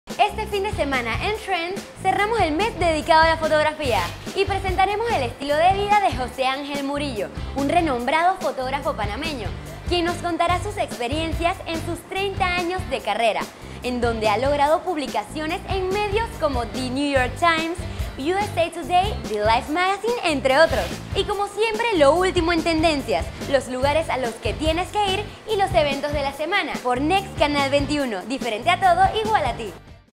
Adelanto prog #30
Este sábado en TRENDS te presentaremos la interesante historia de José Angel Murillo, ademas de tendencias como los  EAR CUFF y unos zapatos no muy convencionales.